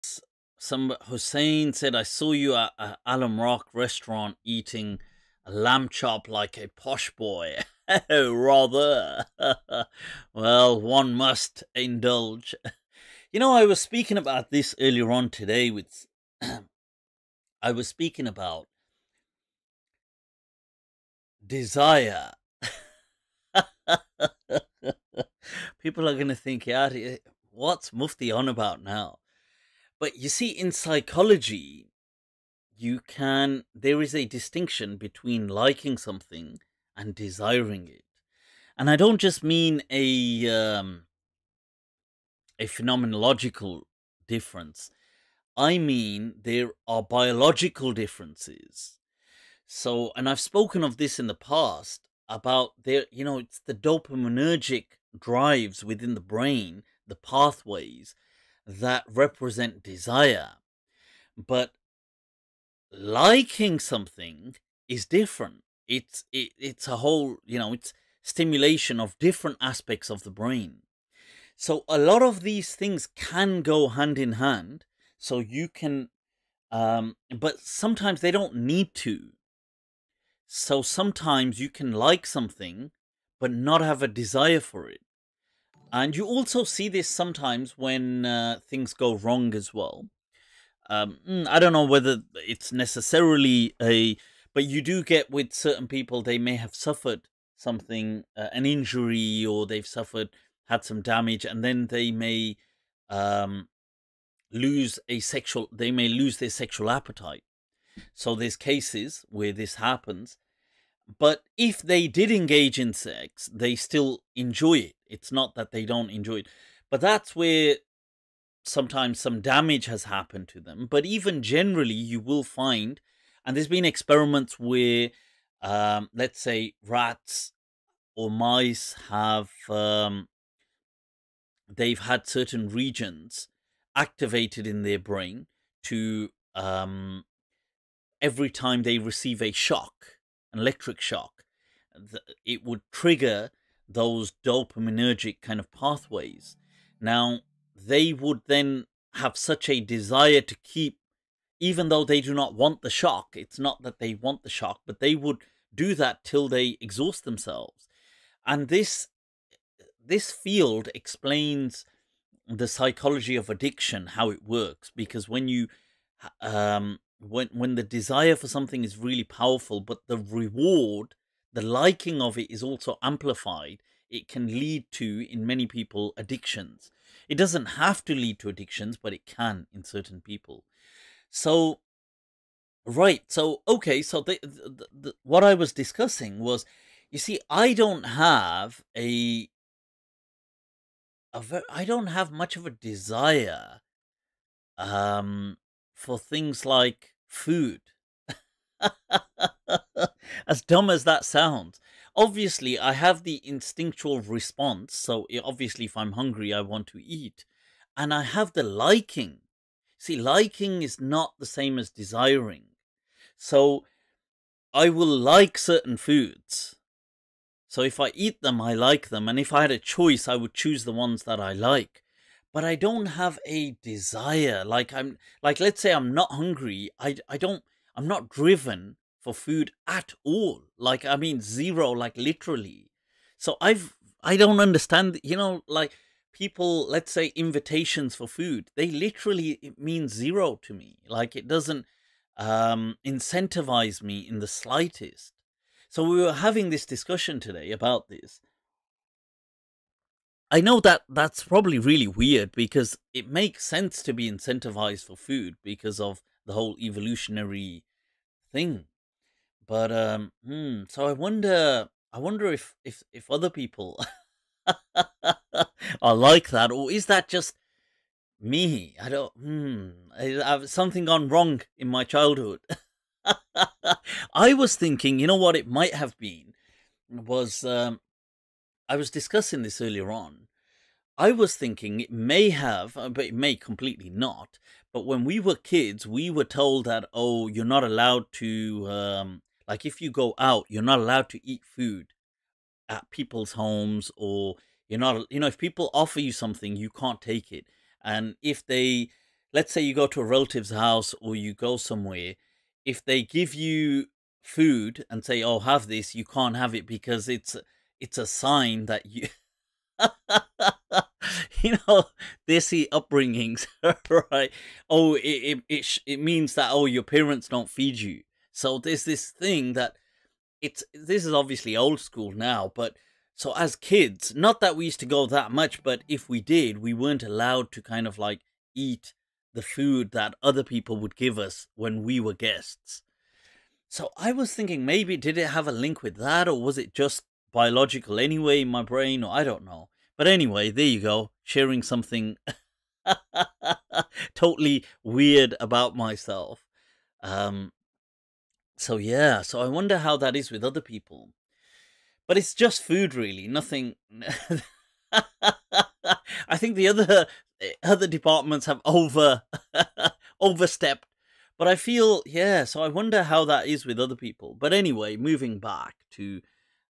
Desire & Liking - A distinction | Mufti Abu Layth
Mufti Abu Layth explains the distinction between desire and liking.